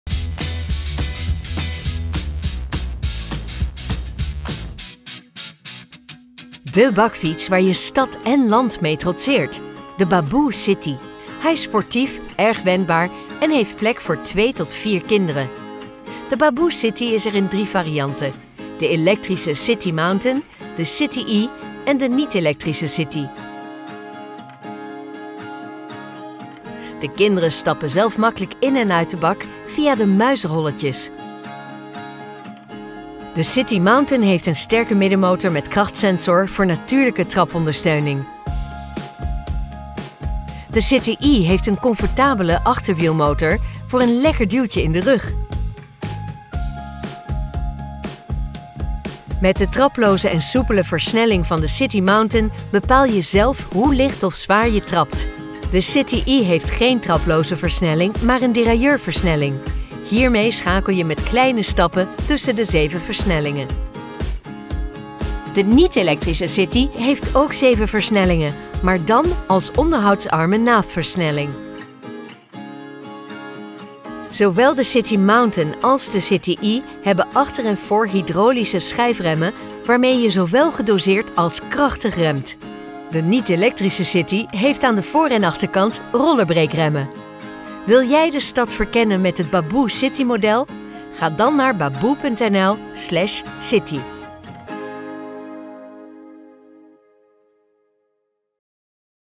Babbaoe City tweewieler bakfiets
De Babboe City is een wendbare tweewielbakfiets met een sportief karakter. Dankzij de slanke vormgeving is deze bakfiets erg praktisch op drukkere wegen. Je manoeuvreert gemakkelijk door het verkeer, zelfs met een volle bak. Bezoek onze winkel in Kampen en maak vrijblijvend een testrit.